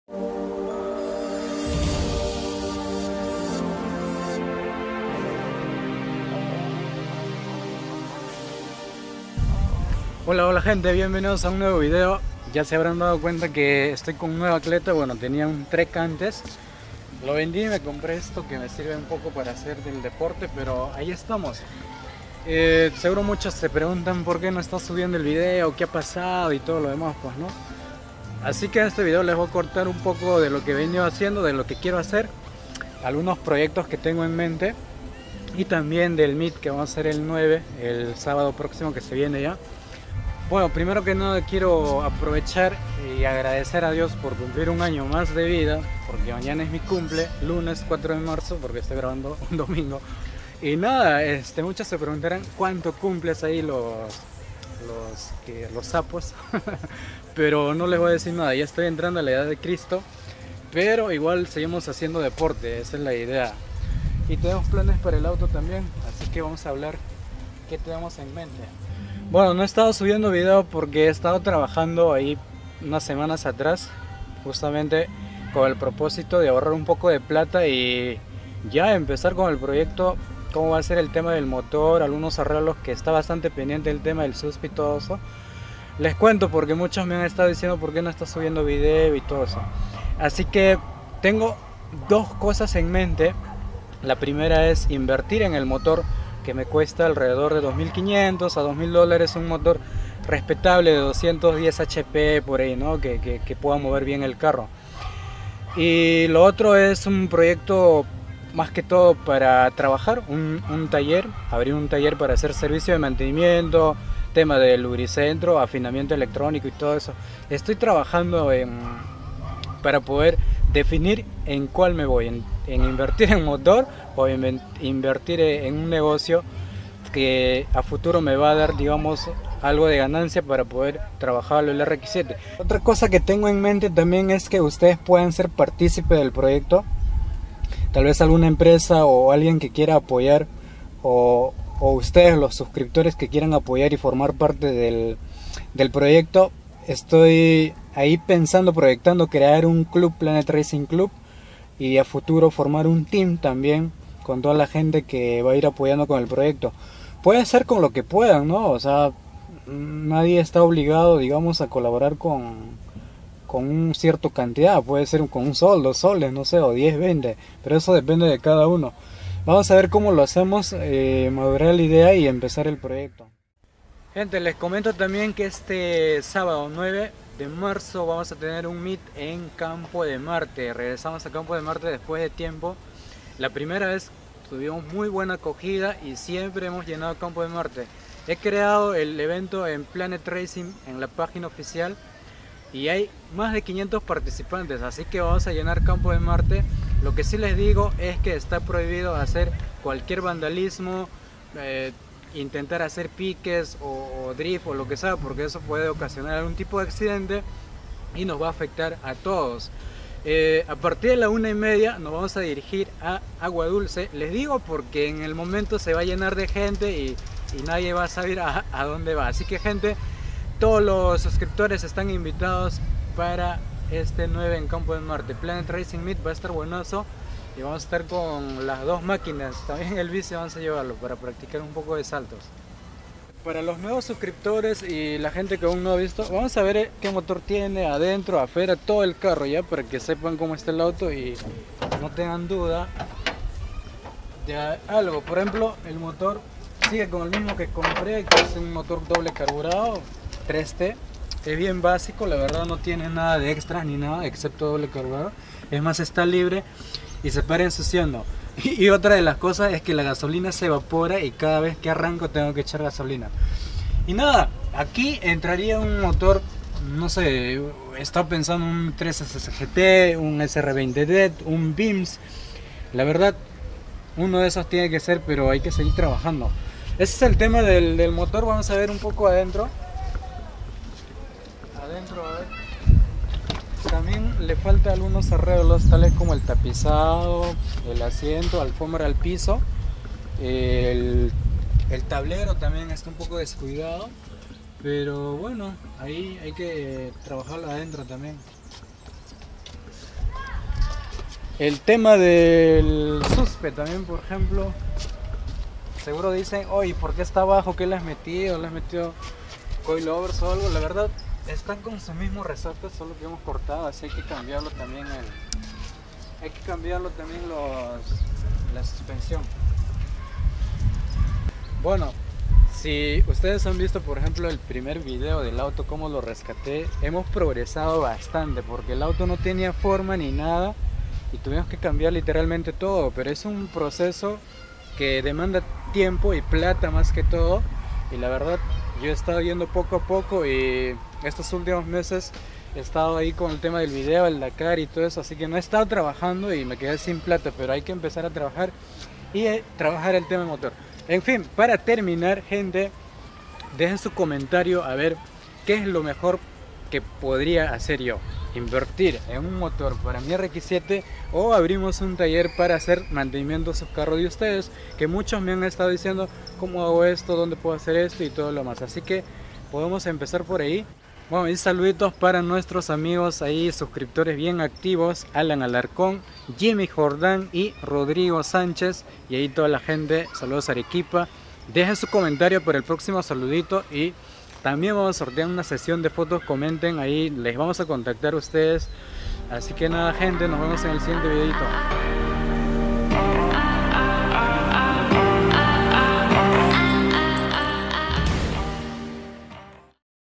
🚨QUE TENGO PENSADO PARA ESTE AÑO? | RX7 FC | 2019🚘
Hola amigos! volviendo al rx7 tengo muchas cosas en mente para este año. una de las cosas mas importates es el motor. vamos como nos va..